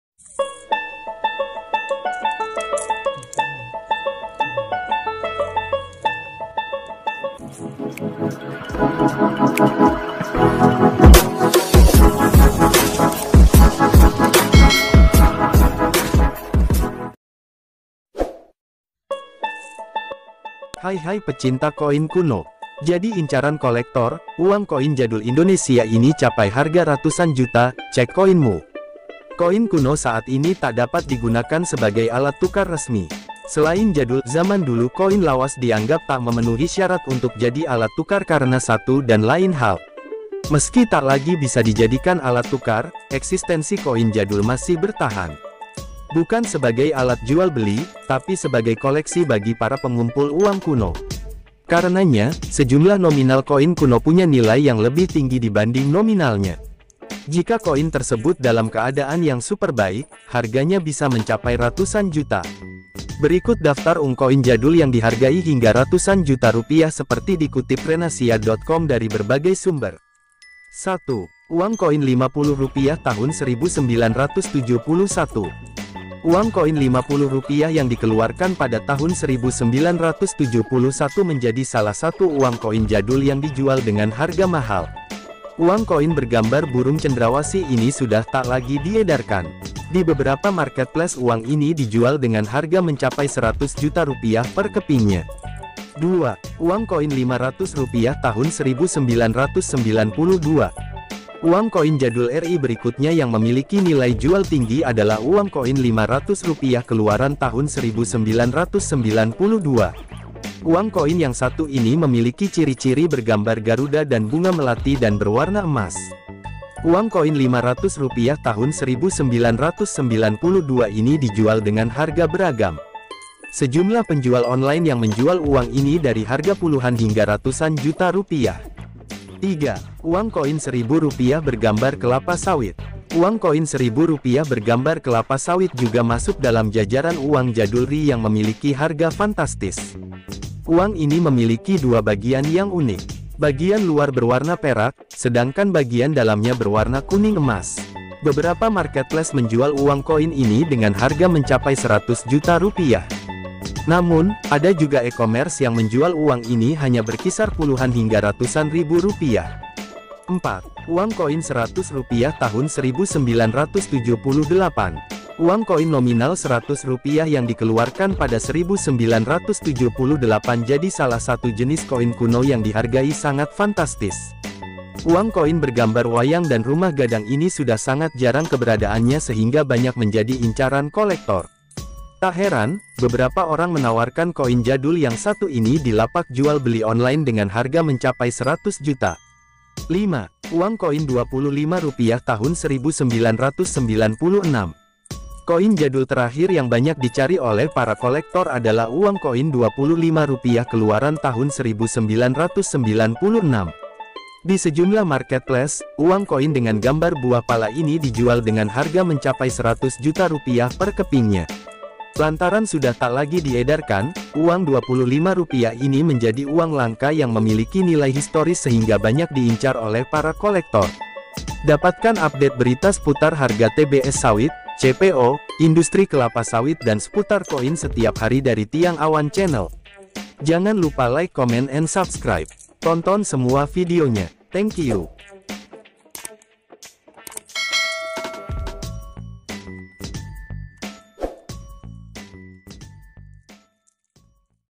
JADI INCARAN KOLEKTOR, KOIN JADUL INDONESIA CAPAI HARGA RATUSAN JT
HAI HAI PECINTA KOIN KUNO,

JADI Incaran Kolektor, Uang Koin Jadul Indonesia Ini Capai Harga Ratusan Juta, CEK KOIN MU.
 Koin kuno saat ini tak dapat digunakan sebagai alat tukar resmi. Selain jadul (jaman dulu), koin lawas dianggap tak memenuhi syarat untuk jadi alat tukar karena satu dan lain hal.

Meski tak lagi bisa dijadikan alat tukar, eksistensi koin jadul masih bertahan. Bukan sebagai alat jual beli, tapi sebagai koleksi bagi para pengumpul uang kuno.

Karenanya, sejumlah nominal koin kuno punya nilai yang lebih tinggi dibanding nominalnya. Jika koin tersebut dalam keadaan yang super baik, harganya bisa mencapai ratusan juta.

Berikut daftar ung koin jadul yang dihargai hingga ratusan juta rupiah seperti dikutip TrenAsia.com dari berbagai sumber.

1. Uang Koin Rp50 Tahun 1971.
Uang koin Rp50 yang dikeluarkan pada tahun 1971  menjadi salah satu uang koin jadul yang dijual dengan harga mahal.

Uang koin bergambar burung cendrawasih ini sudah tak lagi diedarkan. Di beberapa marketplace uang ini dijual dengan harga mencapai Rp100 juta per kepingnya.

2. Uang Koin Rp500 Tahun 1992
Uang koin jadul R I berikutnya yang memiliki nilai jual tinggi adalah uang koin Rp500 keluaran tahun 1992.

Uang koin yang satu ini memiliki ciri-ciri bergambar garuda dan bunga melati dan berwarna emas. Uang koin Rp500 tahun 1992 ini dijual dengan harga beragam.

Sejumlah penjual online yang menjual uang ini dari harga puluhan hingga ratusan juta rupiah.

3. Uang Koin Rp1.000 Bergambar Kelapa Sawit.

Uang koin Rp1.000 bergambar kelapa sawit juga masuk dalam jajaran uang jadul RI yang memiliki harga fantastis. Uang ini memiliki dua bagian yang unik.

Bagian luar berwarna perak, sedangkan bagian dalamnya berwarna kuning emas. Beberapa marketplace menjual uang koin ini dengan harga mencapai 100 juta rupiah.

Namun, ada juga ecommerce yang menjual uang ini hanya berkisar puluhan hingga ratusan ribu rupiah.

4. Uang Koin Rp100 Tahun 1978.
Uang koin  nominal Rp100 yang dikeluarkan pada 1978 jadi salah satu jenis koin kuno yang dihargai sangat fantastis.

Uang koin bergambar wayang dan rumah gadang ini sudah sangat jarang keberadaannya sehingga banyak menjadi incaran kolektor.

Tak heran, beberapa orang menawarkan koin jadul yang satu ini di lapak jual beli online dengan harga mencapai 100 juta.

5. Uang Koin Rp25 Tahun 1996
Koin jadul terakhir yang banyak dicari oleh para kolektor adalah uang koin Rp25 keluaran tahun 1996.

Di sejumlah marketplace, uang koin dengan gambar buah pala ini dijual dengan harga mencapai Rp100 juta per kepingnya.

Lantaran sudah tak lagi diedarkan, uang Rp25 ini menjadi uang langka yang memiliki nilai historis sehingga banyak diincar oleh para kolektor.
Koin ini mahal

Dapatkan apdet berita seputar harga TBS Sawit, CPO, industri kelapa sawit dan seputar koin setiap hari dari TIANG AWAN CHANNEL.
tonton semua videonya.
Tengkiu